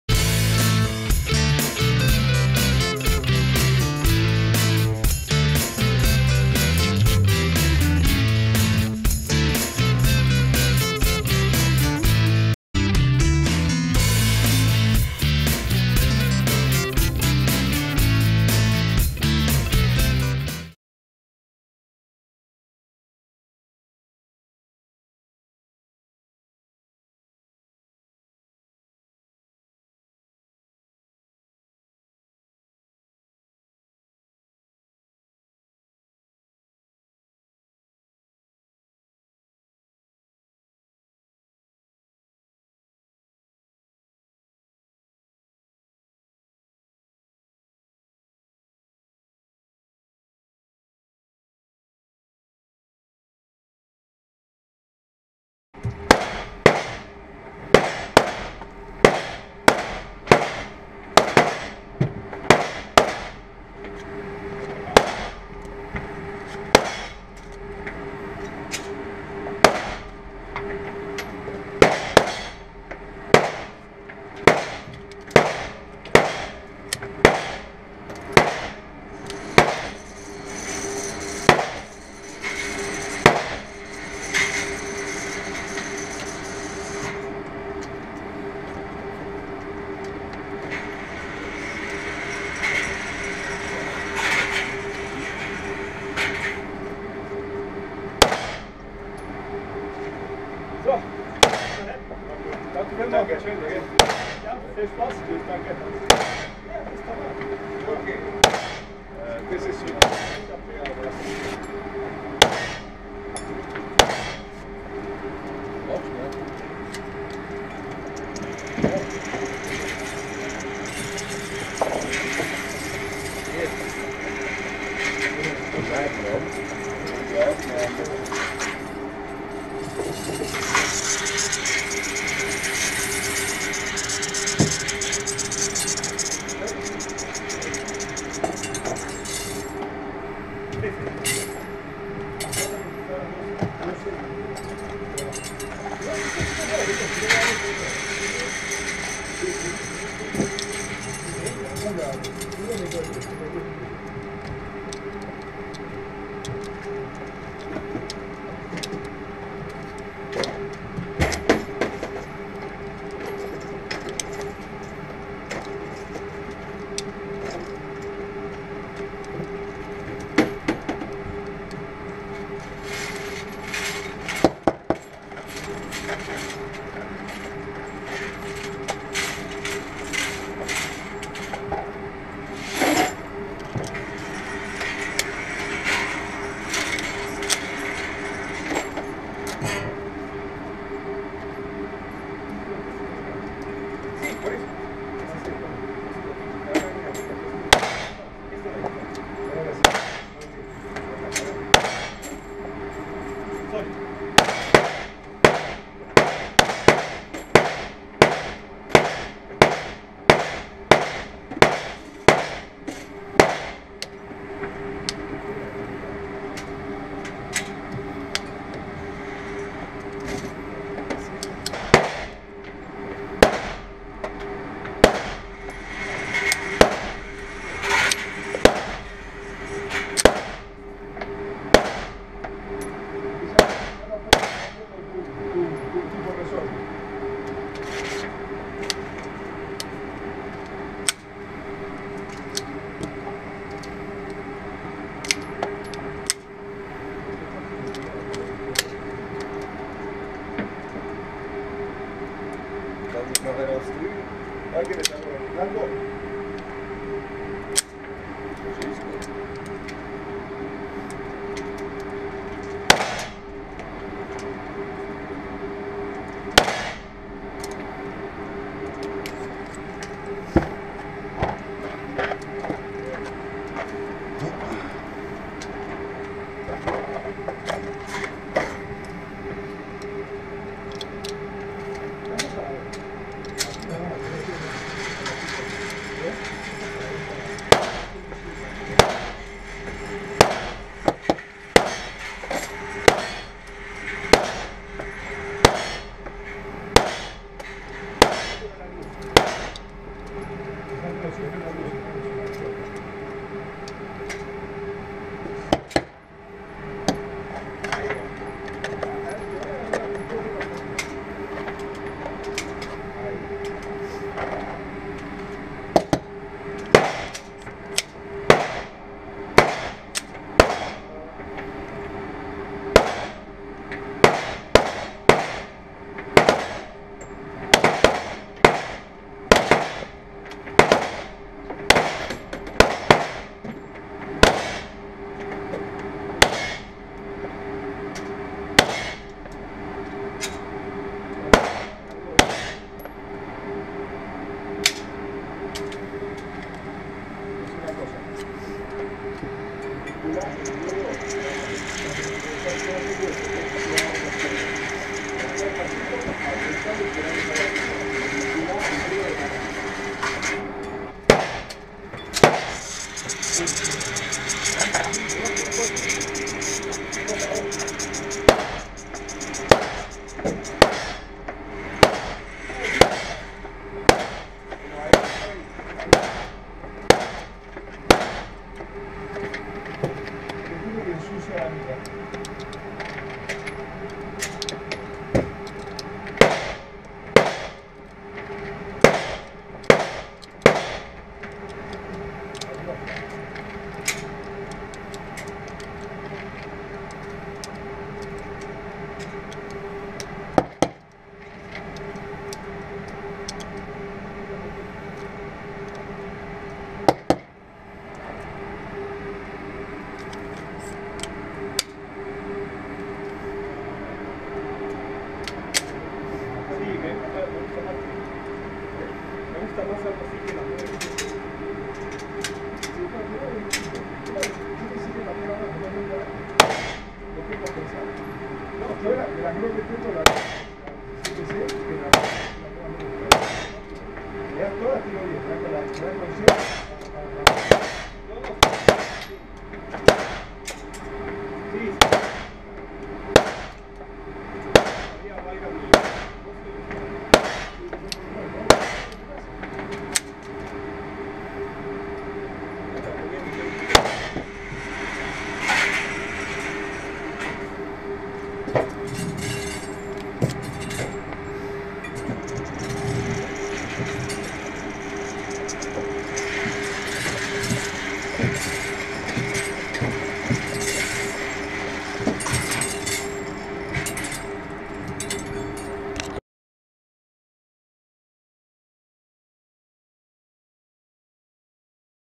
SIG P210 (ex swiss army) range shooting
Prova a fuoco in poligono al chiuso di una Sig P 210 ex ordinanza esercito svizzero con numero di serie A (Armee')
La prova viene effettuata con munizioni commeriali di marche diverse, tutte piu' o meno dello stesso peso, ad una distanza di circa 15/18 metri . Ricordandovi di sottoscrivere al mio canale, di cliccare "mi piace" e di condividere i contenuti, vi auguro una buona visione.       A presto